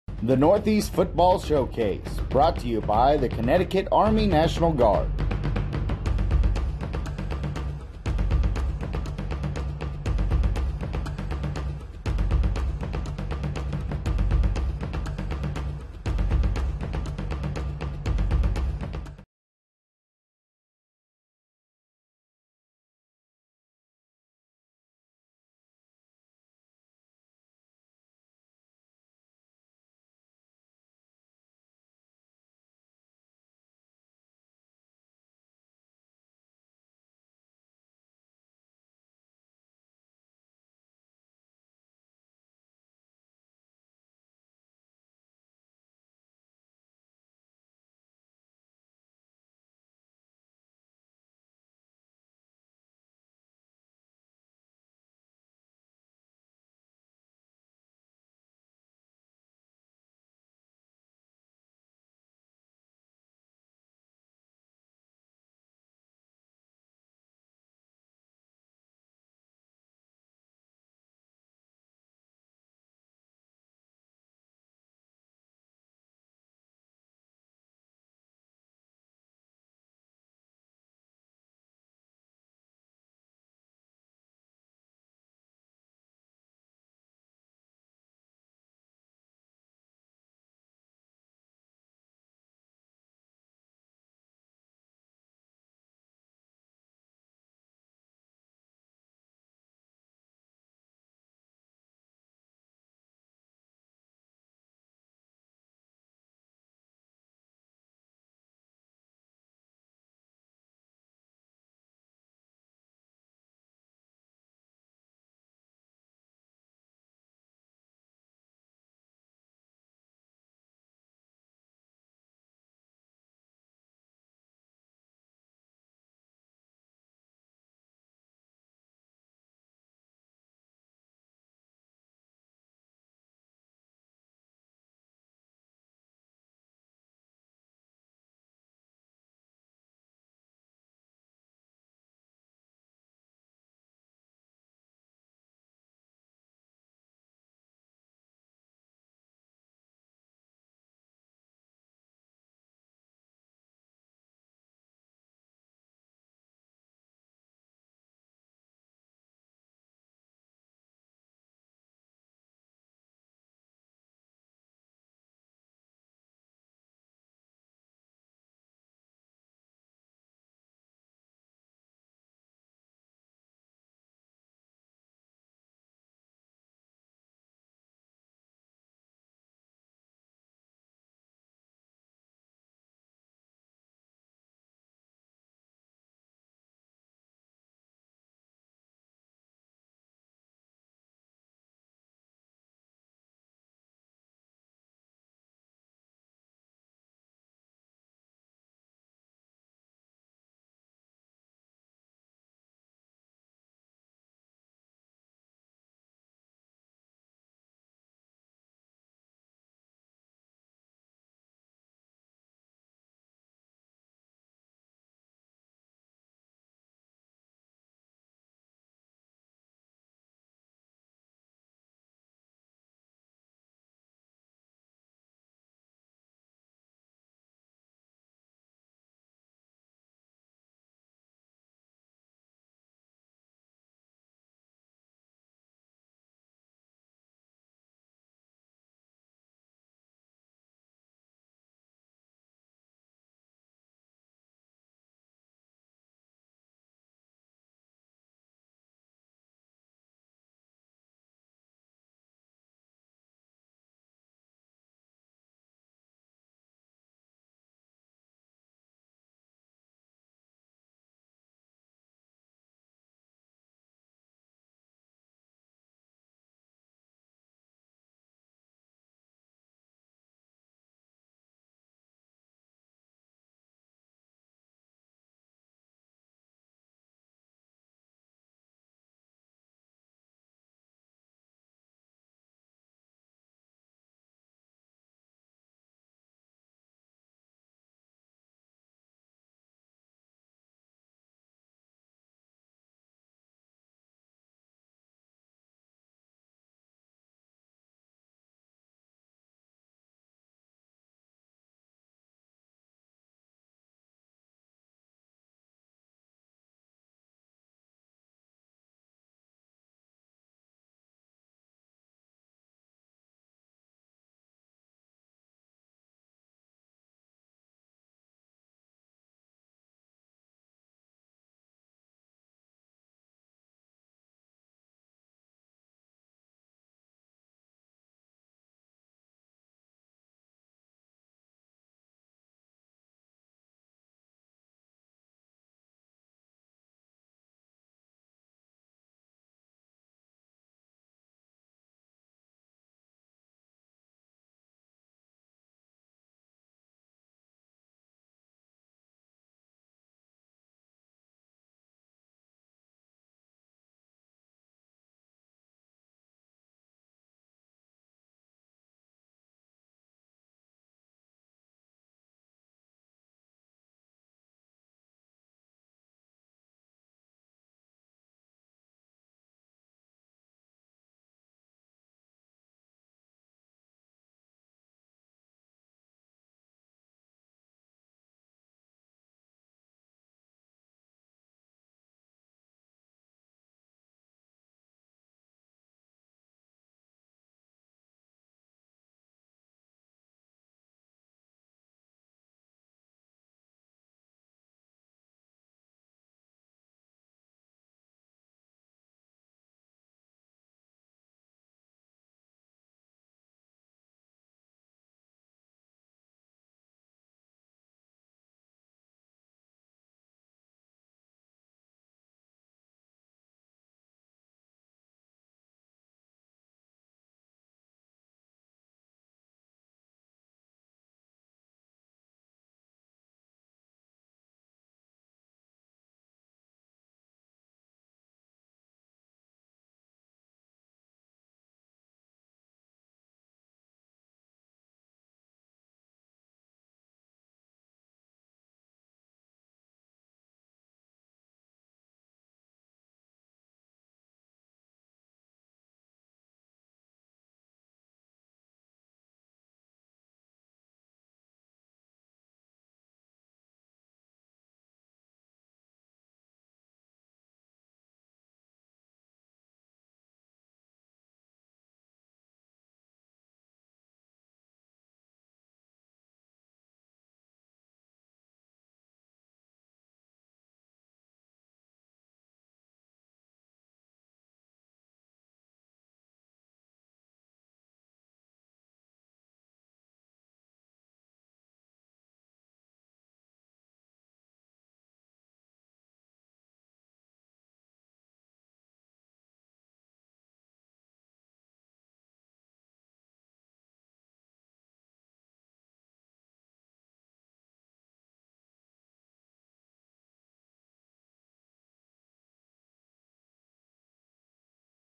The 2023 Northeast Football Showcase, featuring JORDAN MILLER #149
Bench: 135 lbs, 14 reps
40-yd: 5.61 secs
Shuttle: 4.78 secs
3 Cone Drill: 7.79 secs

The annual Showcase takes place each spring in Connecticut and allows high school football players an opportunity to display their skills for college coaches as the athletes look to move up to the next level. To have your games professionally filmed or broadcast LIVE this season, contact Memory to Video Productions at or or visit the